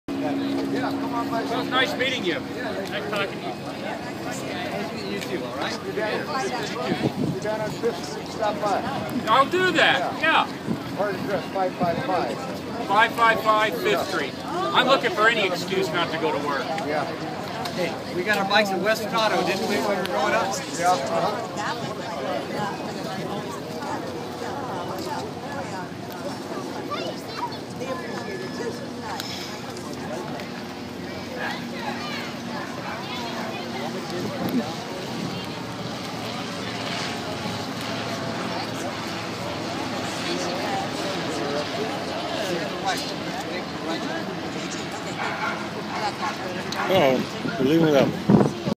USS Iowa Comes Into Harbor
Here is my first video which happens to be the USS Iowa coming into the L A Harbor.